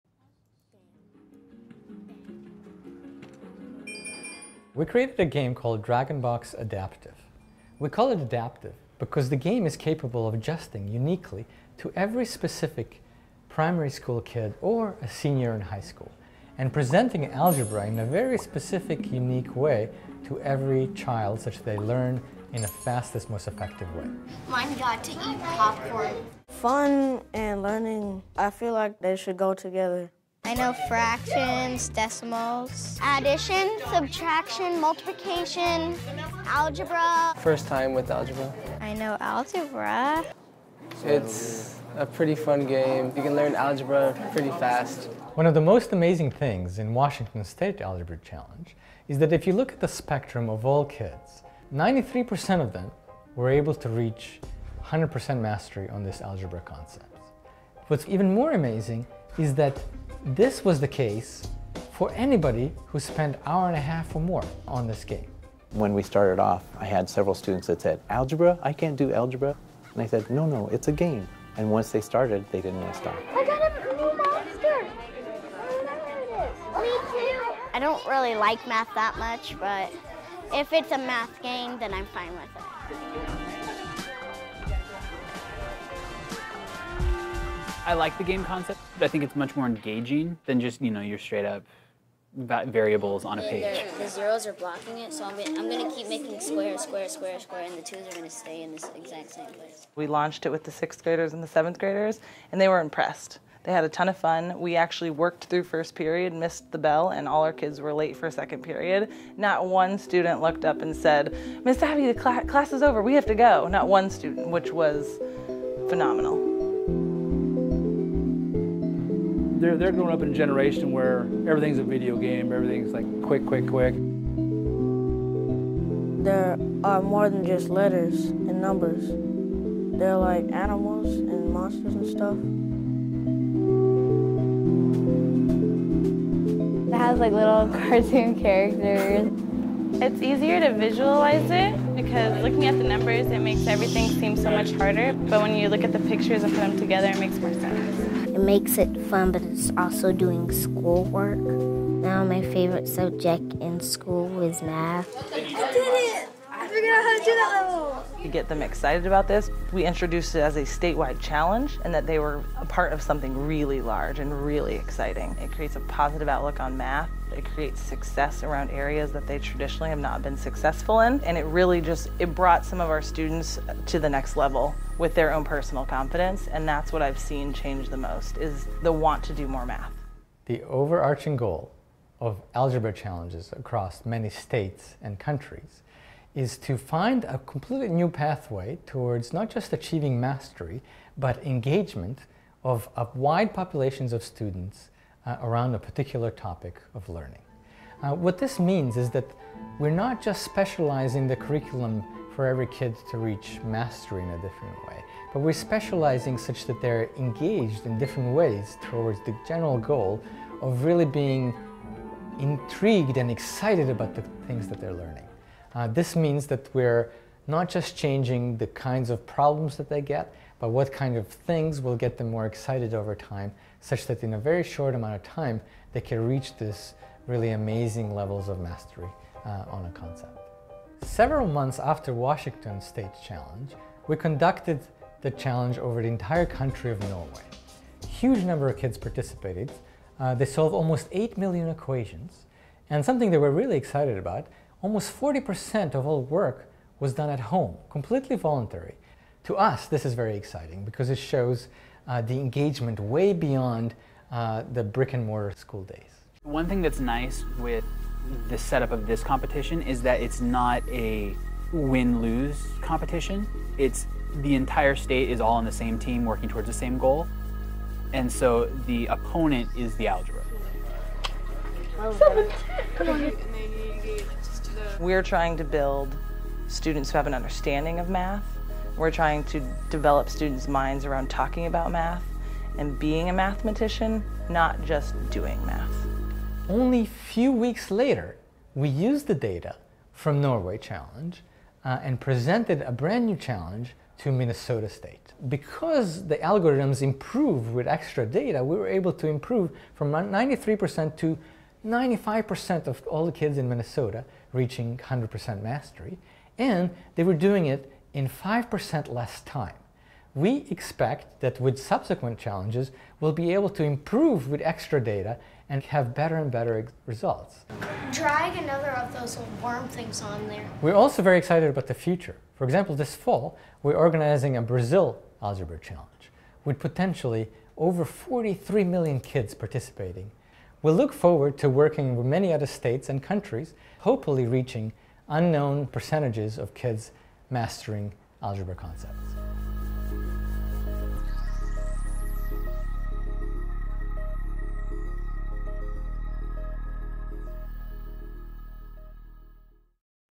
Center for Game Science: DragonBox Adaptive and Alegbra Challenges
Learn more about DragonBox Adaptive, and the success of the Algebra Challenges that have been held so far in Washington, Norway, and Minnesota.

DragonBox Adaptive is a game about the fundamental concepts of algebra. In preliminary trials, it is capable of empowering even a 2nd grader to solve complex algebraic equations (e.g. solve for x) in just 4 hours of play.  As such, it can not only be used as an ideal introduction to equality,and algebra, but also as a powerful remediation tool for struggling high school students.